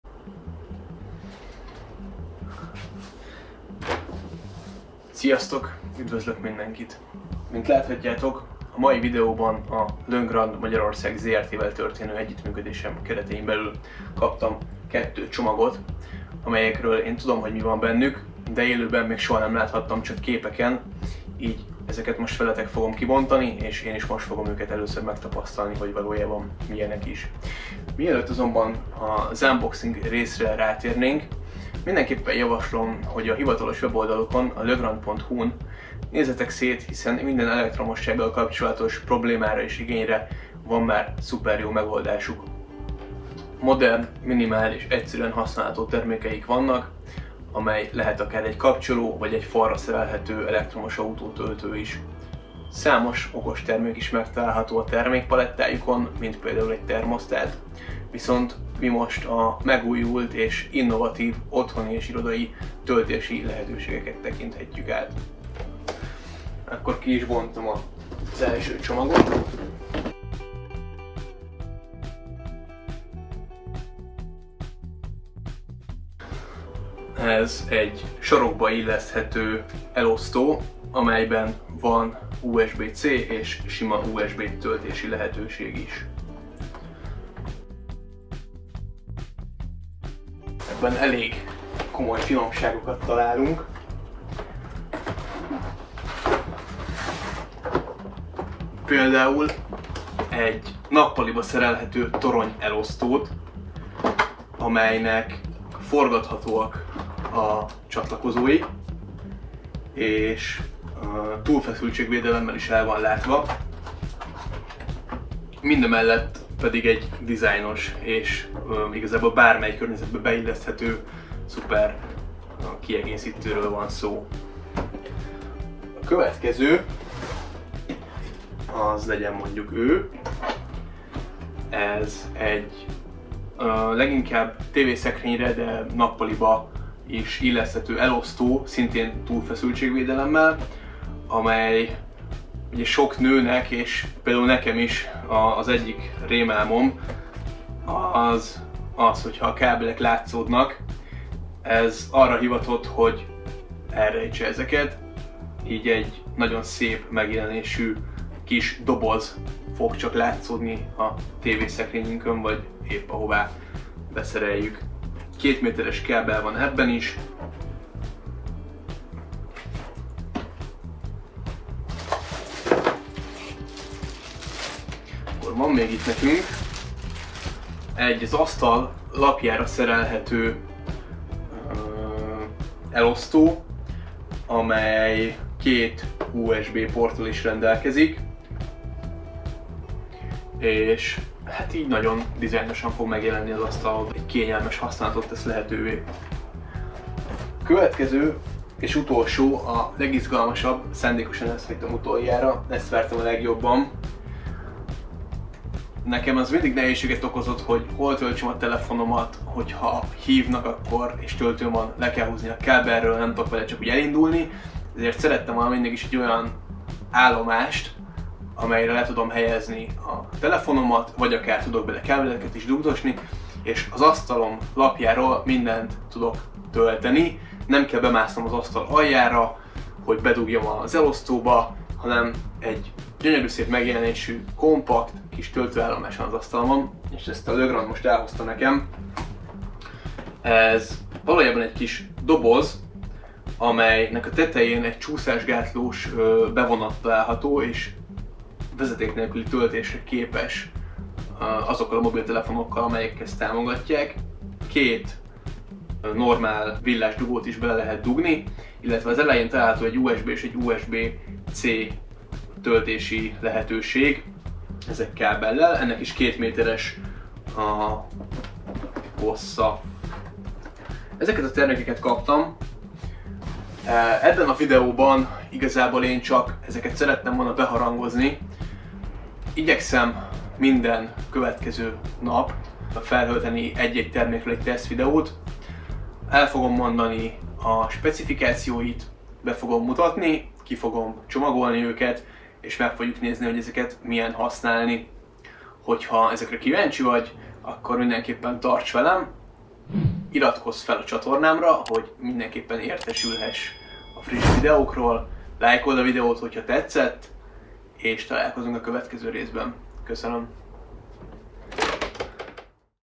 Legrand elosztósorok🔌|Irodai és otthoni prémium elosztók🔋|Túlfeszültség-védelem⚡️|Takács Tamás
A videó a Legrand Magyarország Zrt. együttműködésével valósult meg.
Ezúton is köszönet a termékekért!

Ez egy promóciós videó, amellyel a soron következő Legrand termékteszteket szeretném népszerűsíteni.